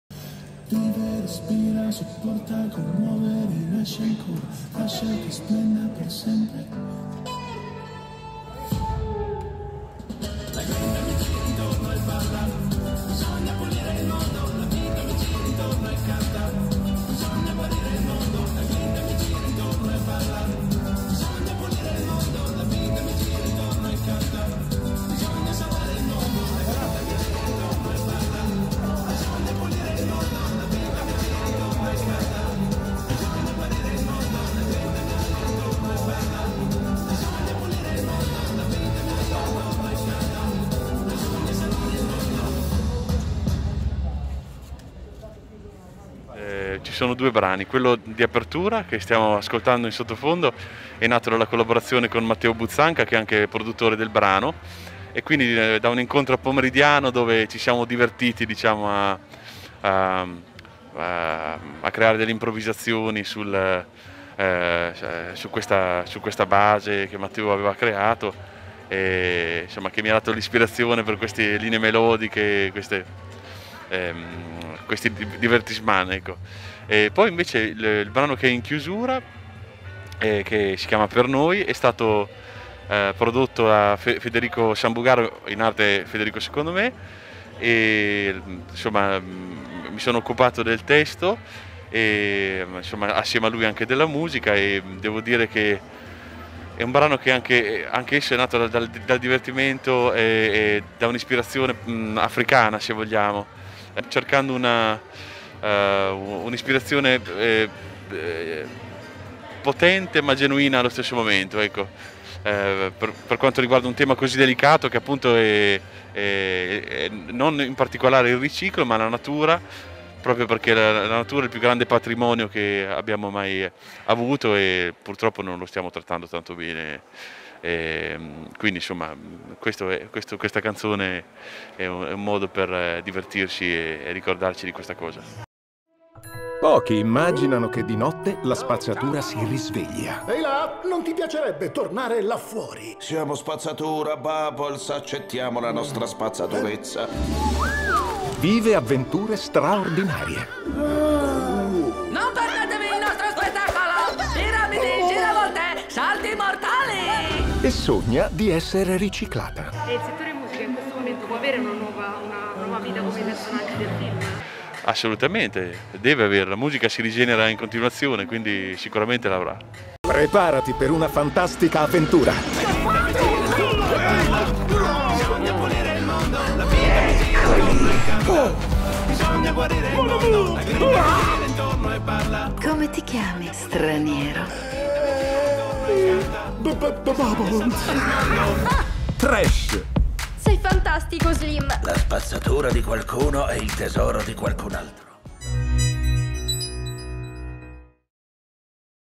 Trash, l'intervista a Raphael Gualazzi
Trash è il film d'animazione diretto da Luca Della Grotta e Francesco Dafano e accompagnato dalla musica di Raphael Gualazzi. Abbiamo incontrato l'artista sul red carpet della pellicola, presentata alla 18esima edizione di Alice nella Città. Trash vi aspetta al cinema!